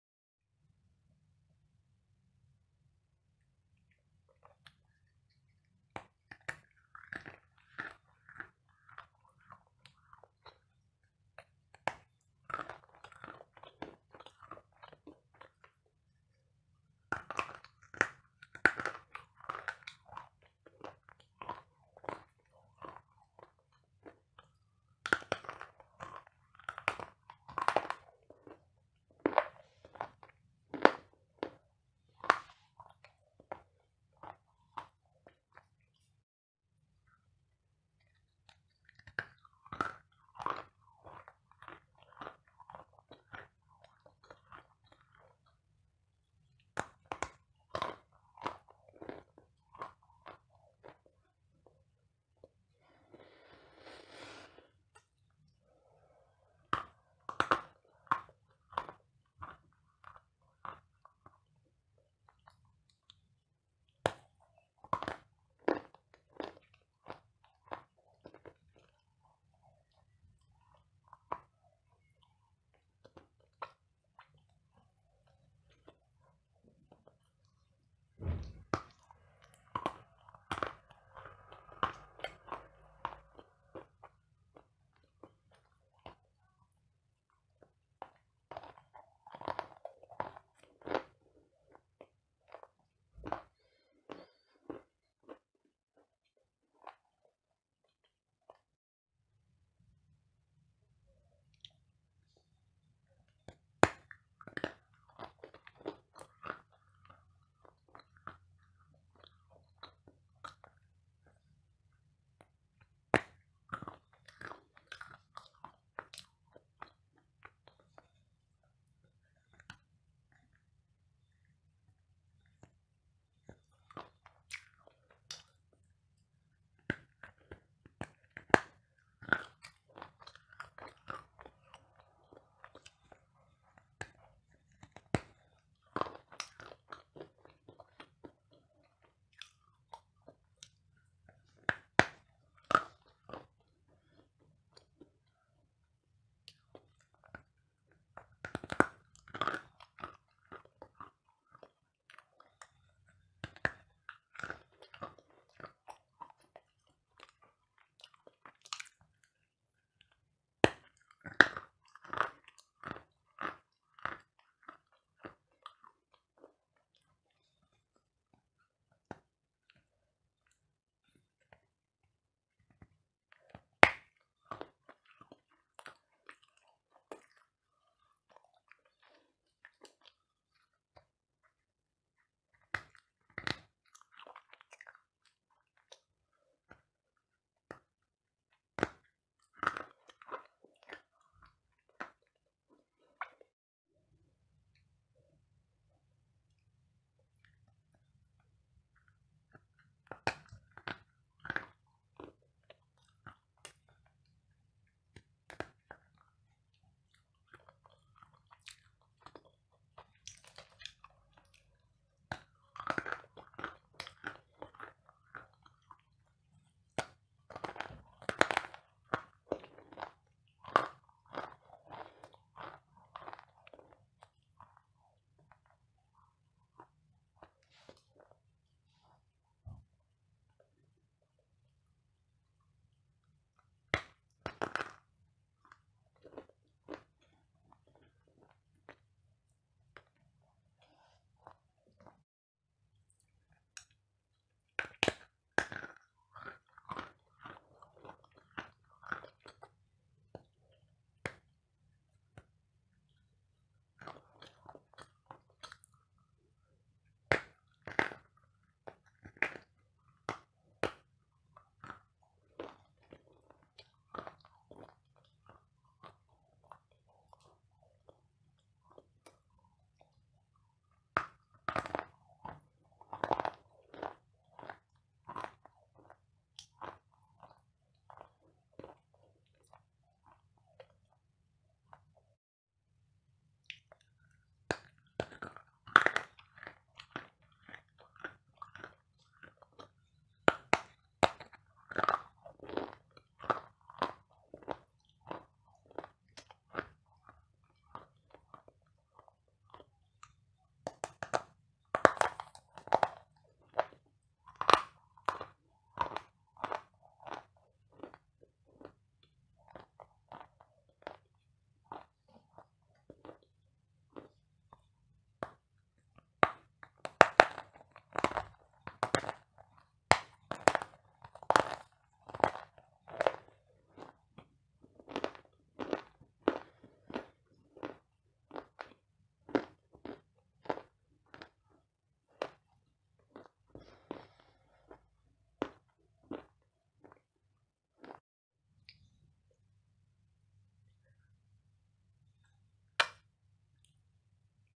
ASMR Marsh Clay clay crunch
Welcome to The Crunchy Kingdom!

Today I munched on Marsh clay. This clay is slowly becoming a favourite of mine. The taste is still like gasoline, the texture is unique, it feels like wet paper that has dried really hard. It's easy to bite into, unless you get a thick chunk like I did. I hope you liked the sounds!

द क्रंची किंगडम में आपका स्वागत है!

आज मैंने मार्श मिट्टी पर चबाया। यह मिट्टी धीरे-धीरे मेरी पसंदीदा होती जा रही है। स्वाद अभी भी गैसोलीन जैसा है, बनावट अद्वितीय है, यह गीले कागज की तरह लगता है जो वास्तव में कठिन सूख गया है। इसे काटना आसान है, जब तक कि आपको मेरे जैसा मोटा हिस्सा न मिले। मुझे आशा है कि आपको ध्वनियाँ पसंद आई होंगी!

मैं मिट्टी/चाक नहीं निगलता, मैं केवल मनोरंजन के उद्देश्य से कुतरता और चबाता हूं। मिट्टी/चाक निगलना आपके स्वास्थ्य के लिए हानिकारक हो सकता है, इसलिए कृपया सावधान रहें!